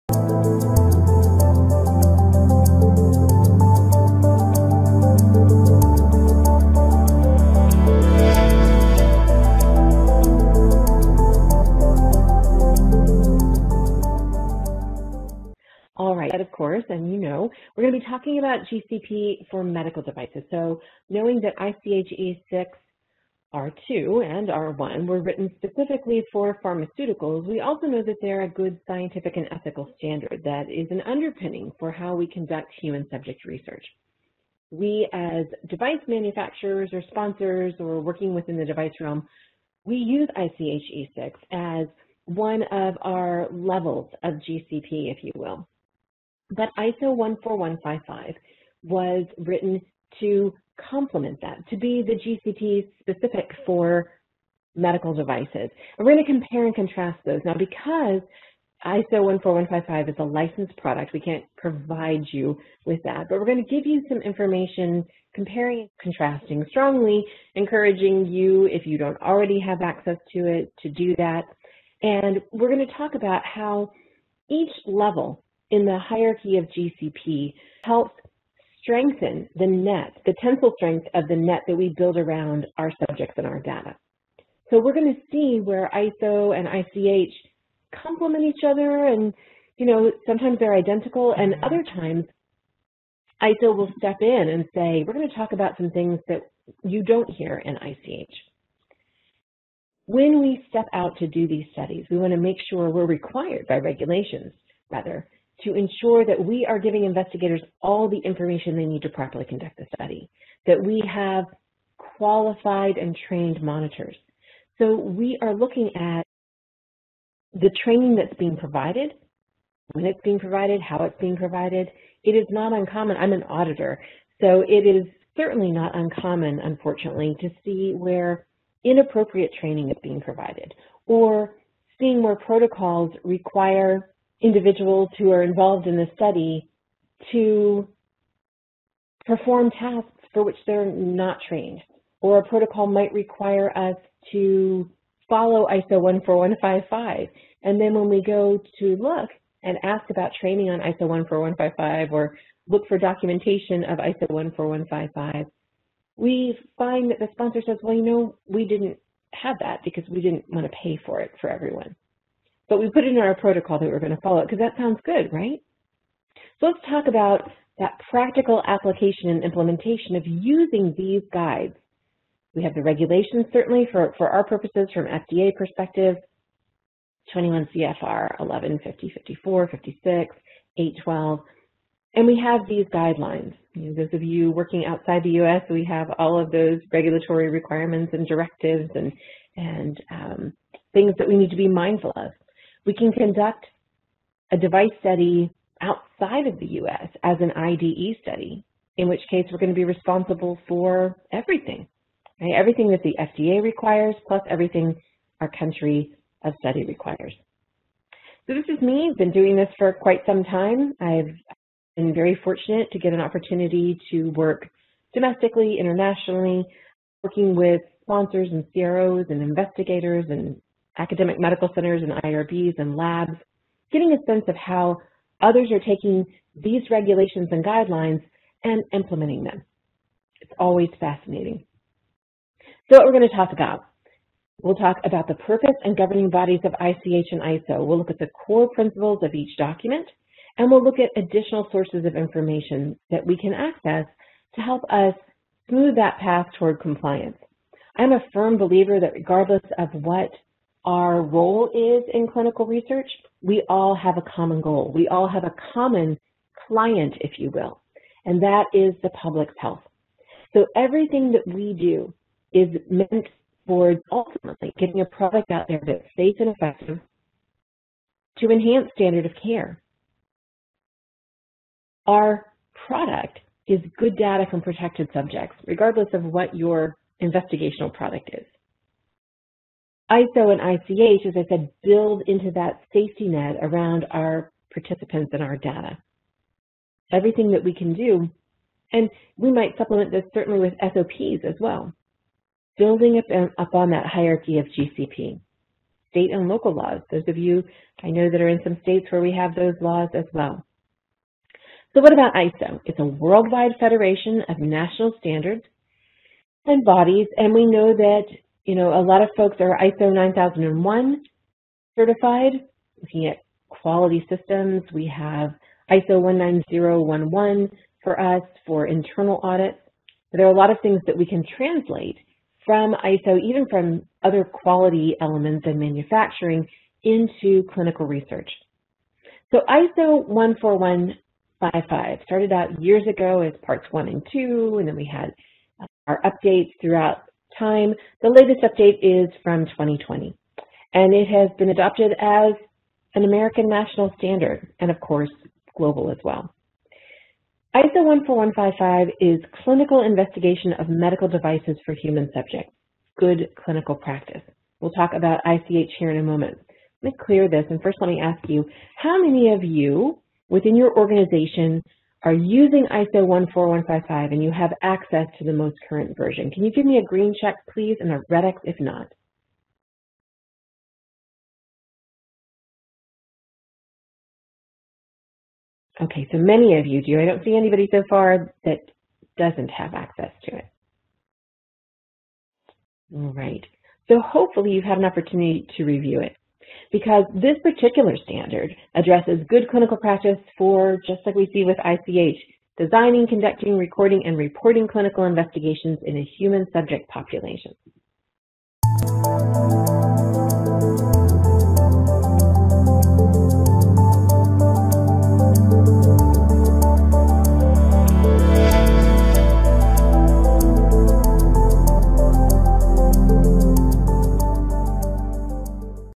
Good Clinical Practice (GCP) for Medical Devices: ICH GCP E6 and ISO 14155
Both the ICH GCP E6 Guideline and ISO 14155 address the elements of Good Clinical Practice in the design, conduct, recording, and reporting of human subject research. Although many sponsors of medical device studies use the ICH GCP E6 Guideline as an ethical and scientific quality standard, ISO is written specifically to protect the rights, safety, and well-being of human subjects, ensure the scientific conduct of the clinical investigation and the credibility of the results, define the responsibilities of the sponsor and Principal Investigator, and assist sponsors, investigators, ethics committees, regulatory authorities and other bodies involved in the conformity assessment of medical devices.